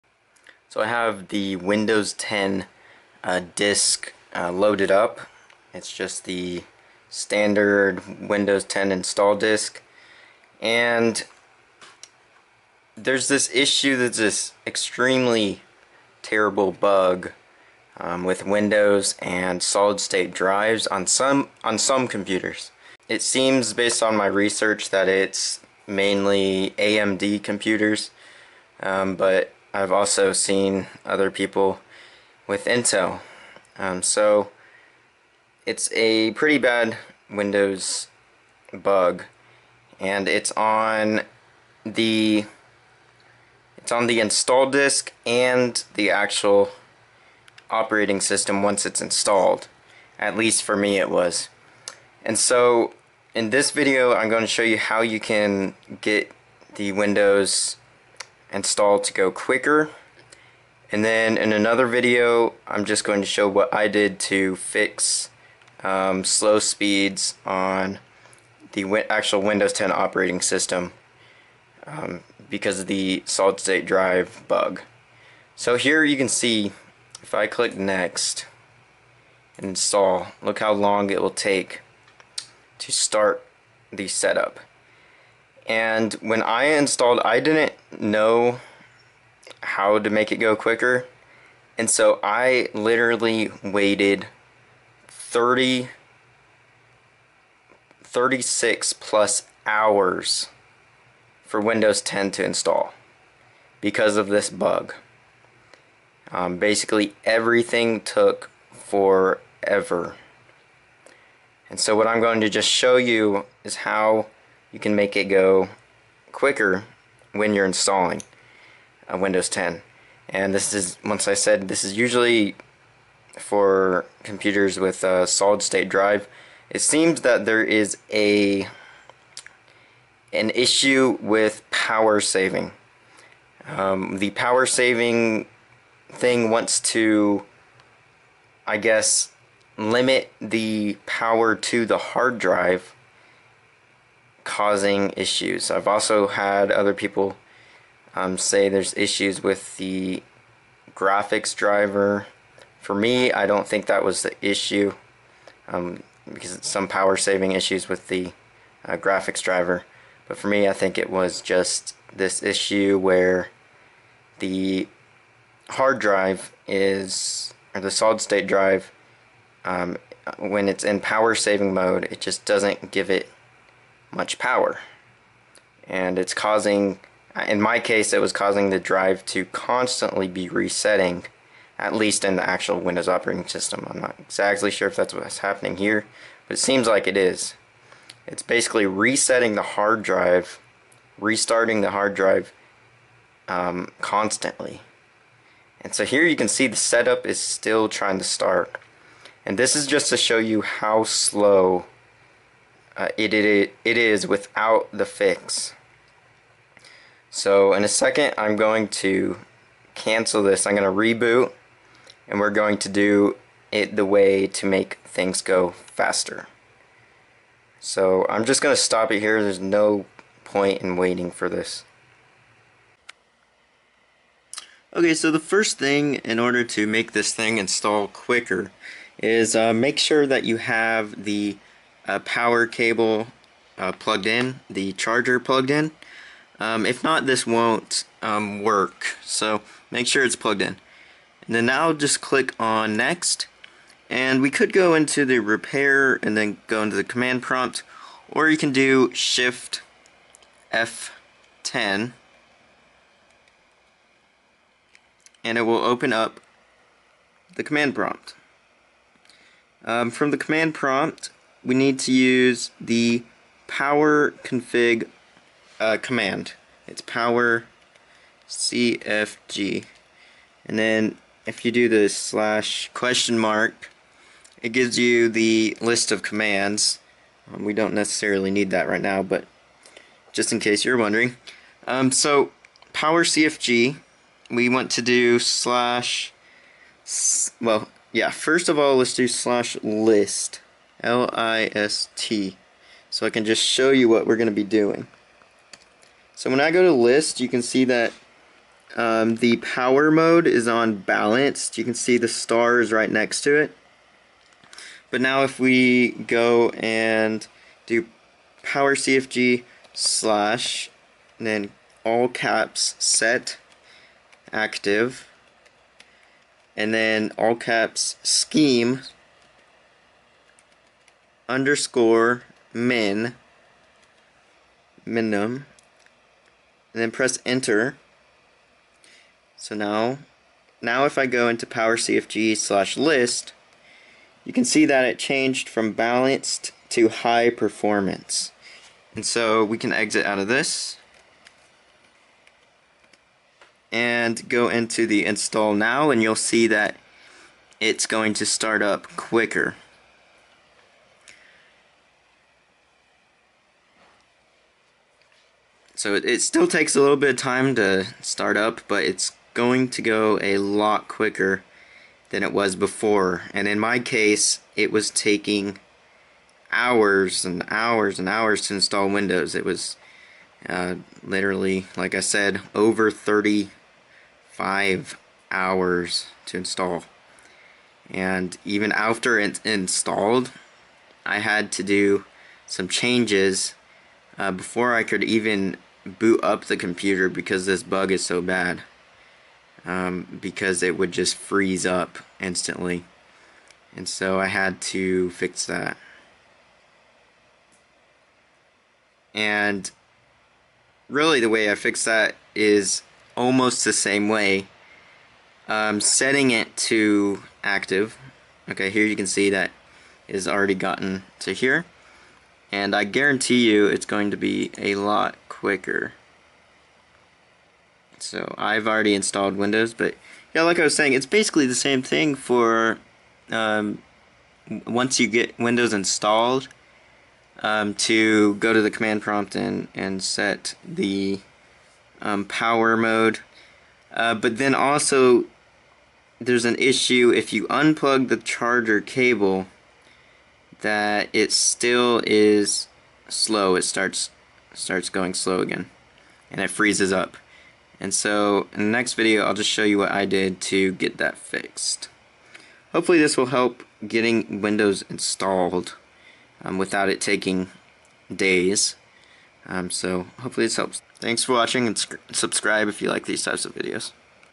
Windows 10 Slow Install on SSD FIX
Windows was taking forever to install and kept freezing up. It seems that on some computers, it takes an extended amount of time to install on a solid state drive. But I now found a solution to speed up the install process on an SSD. This could work for Windows 8 as well.

When you boot into the Windows 10 CD, press the next button. Now press "shift F10", which will bring up the CMD. In the CMD, change the power mode to high performance by typing "powercfg /SETACTIVE SCHEME_MIN". Now things should run quicker. Continue on through the install process.